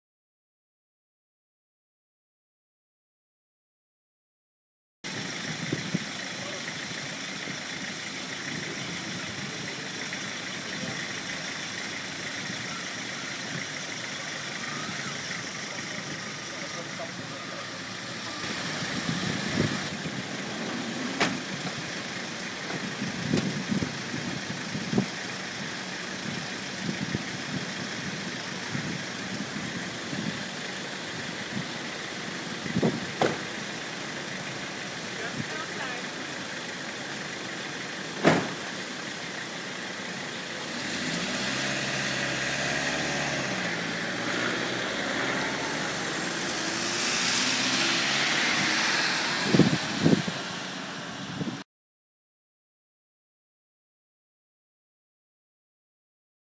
David Cox Leaves Court Nov 6 2012
David Cox leaving Bermuda Appeals Court Nov 6 2012